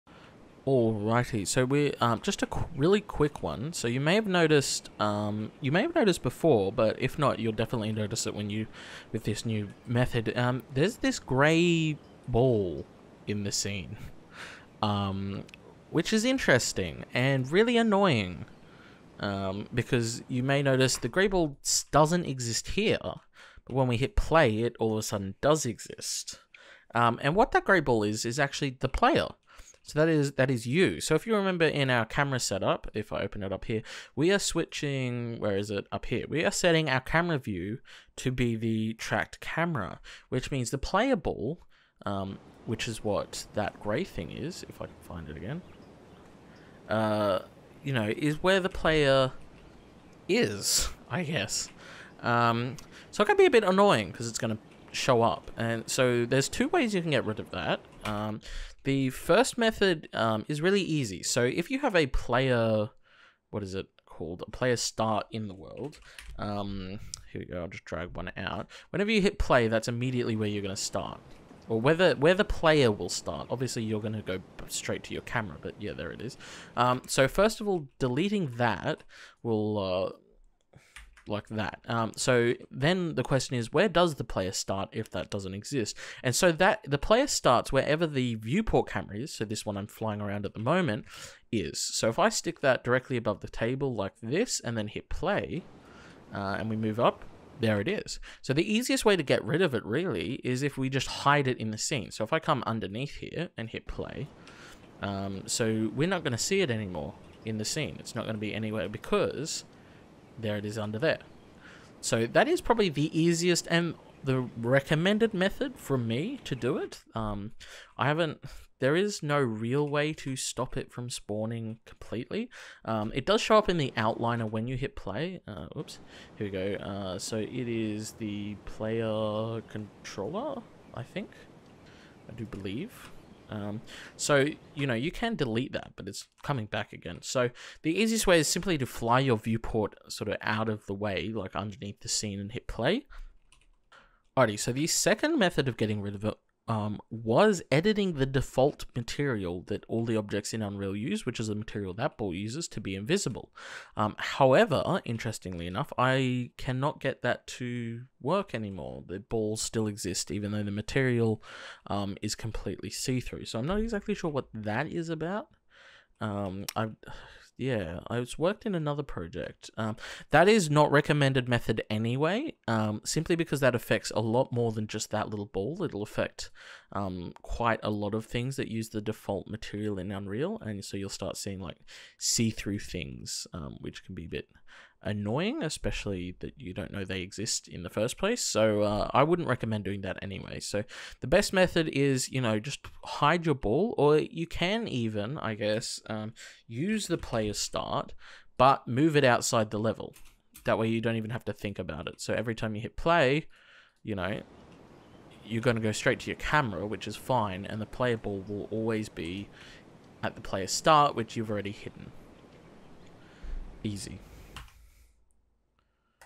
Remove Grey Sphere from Scene in UE4 (Tutorial)
I know some people may be seeing a strange grey sphere in their scene in their virtual production setup, this is an explanation of what it is and how to get rid of it.